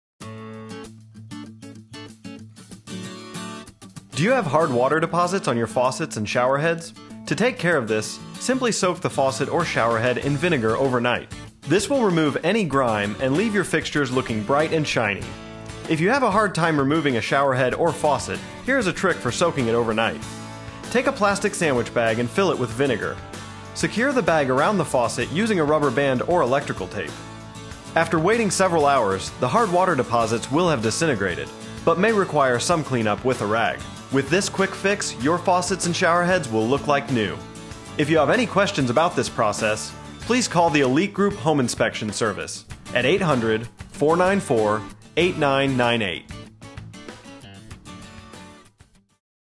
The Elite Group Property Inspections, Inc. - Hard Water Deposits
Hard water can be a nuisance, but just because you are without a water softener doesn't mean you have to live with it! Learn how to remove those pesky stains by watching our video.